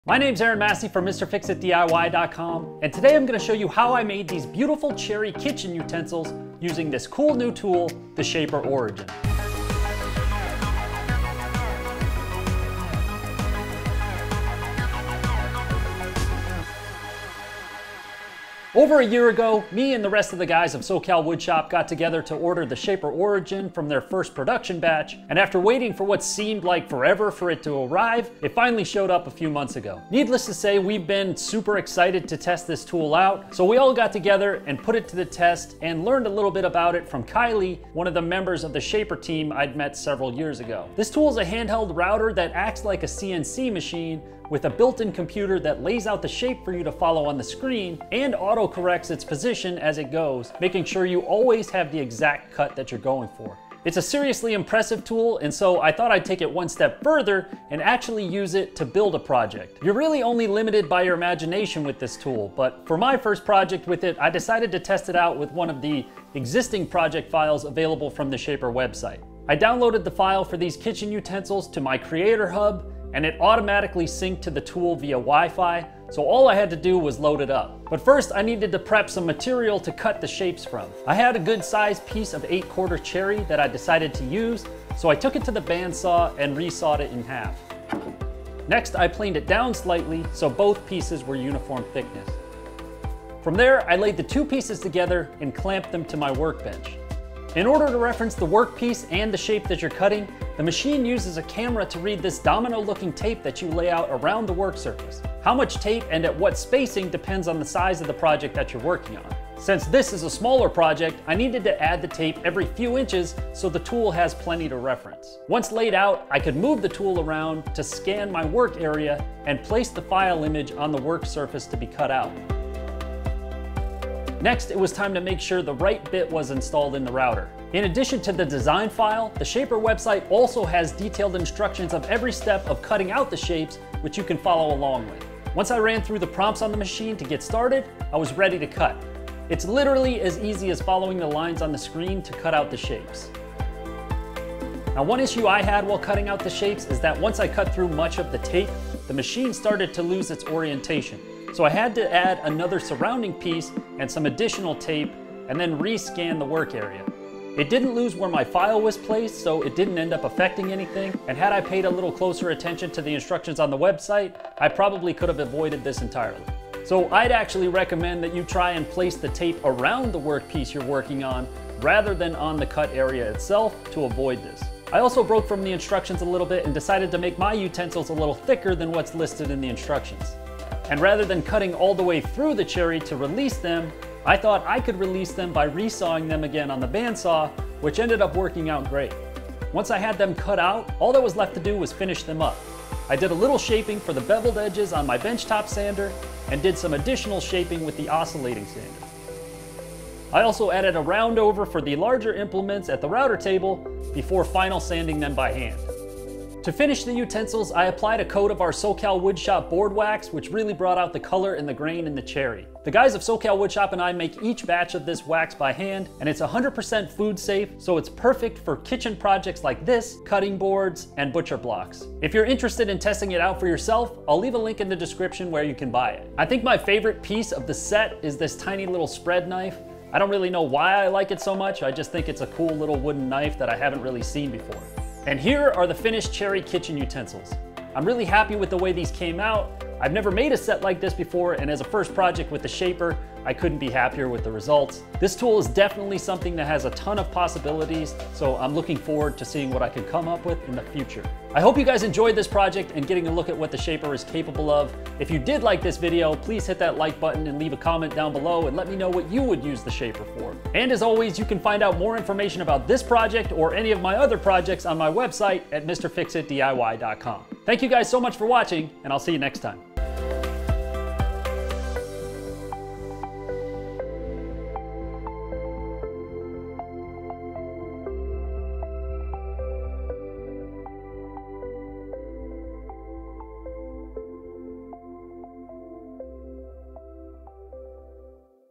Wooden Kitchen Utensils with a Handheld CNC
In this project tutorial and video, I'll show you how I used the Shaper Origin to create a beautiful Cherry wooden kitchen utensil set. This project was my first attempt at using the Shaper Origin and it came out great.